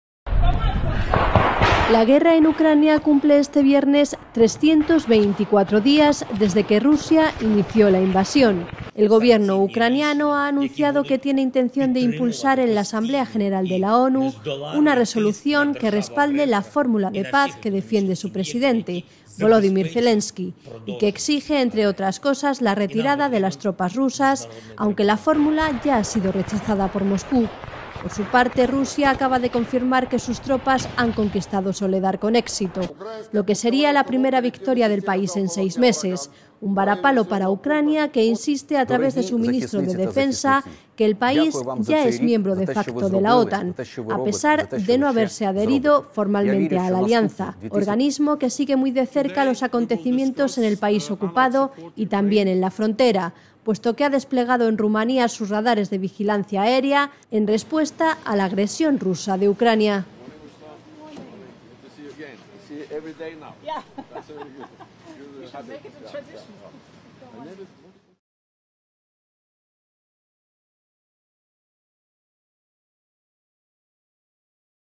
La OTAN despliega en Rumanía sus radares de vigilancia aérea para responder a Rusia
La OTAN ha anunciado el despliegue en Rumanía de aeronaves dotadas con sistemas aerotransportados de alerta y control (AWACS).